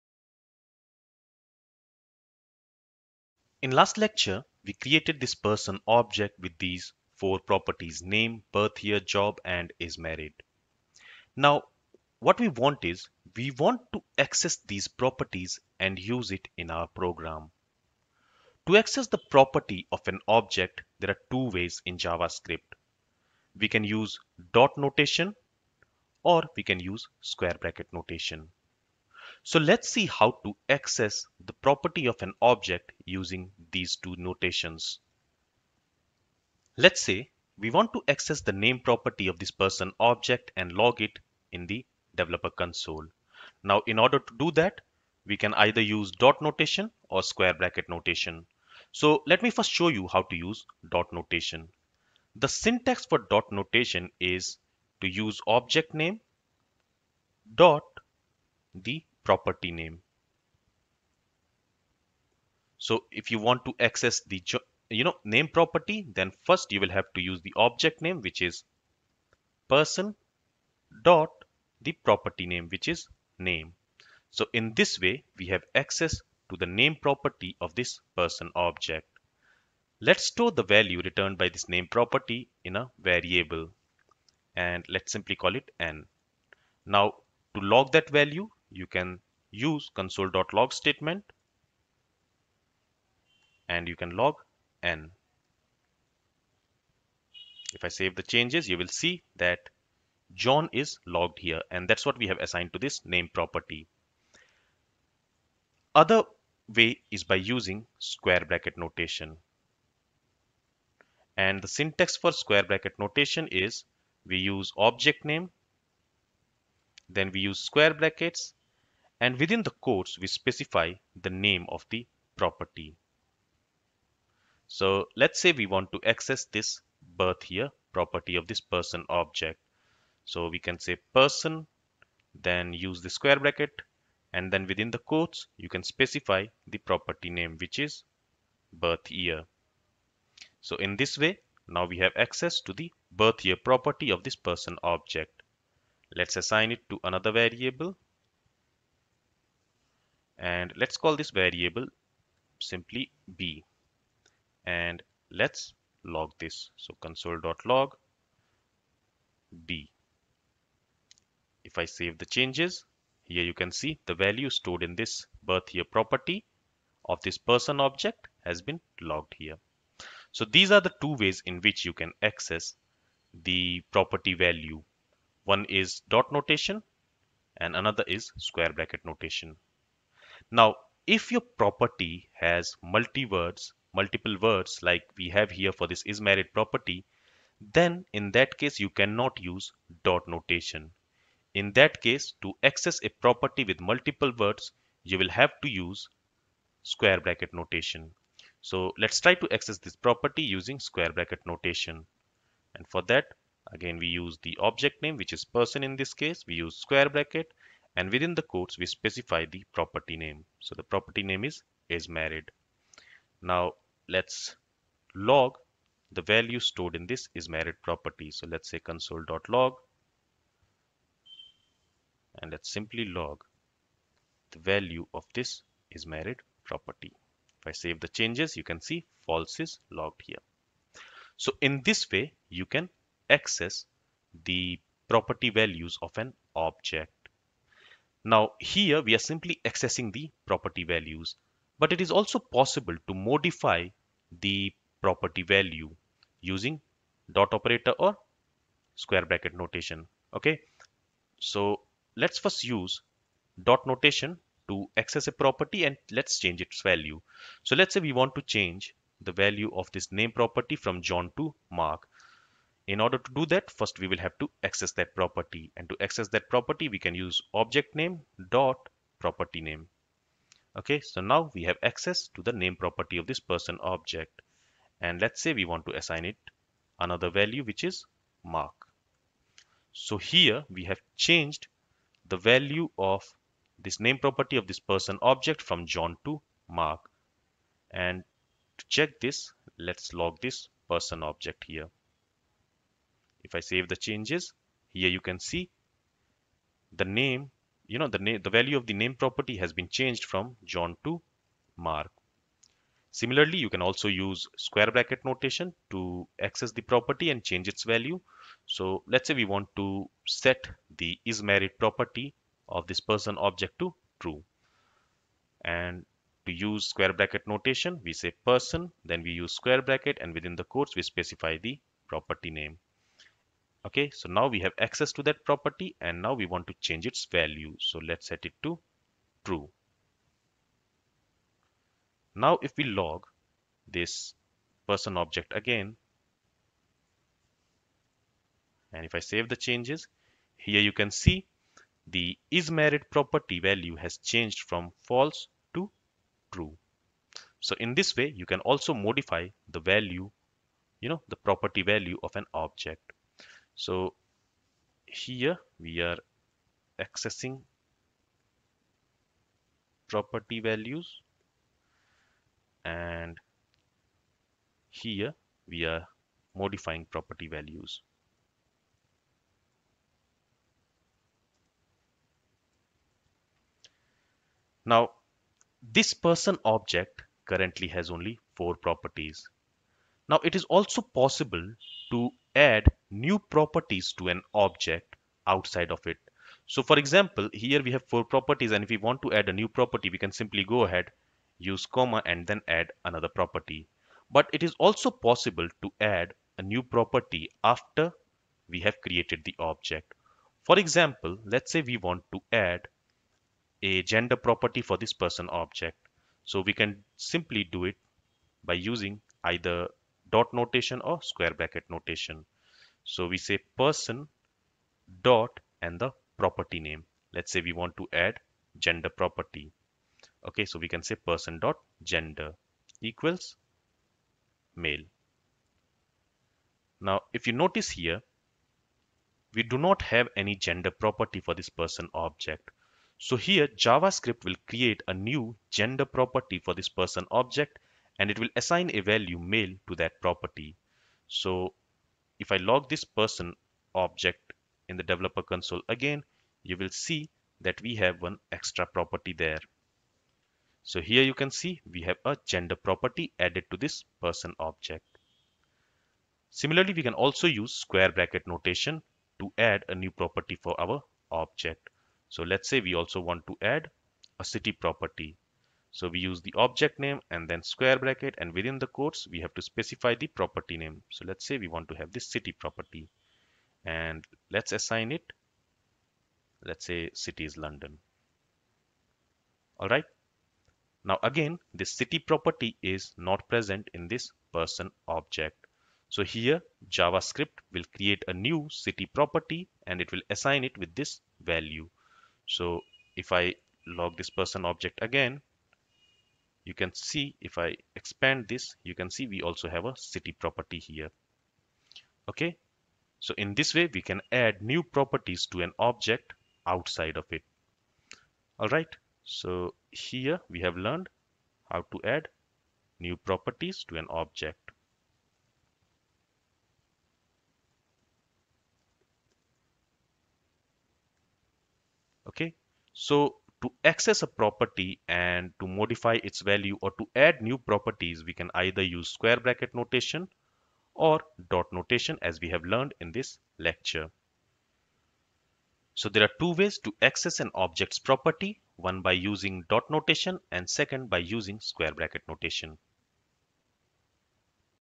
Dot notation vs Square Bracket notation
In this lecture, you will learn how to access objects properties and how to read and manipulate its value. In JavaScript, there are two ways to access and manipulate an objects property:

1. Dot notation
2. Square bracket notation

let's learn about both these syntax in this lecture.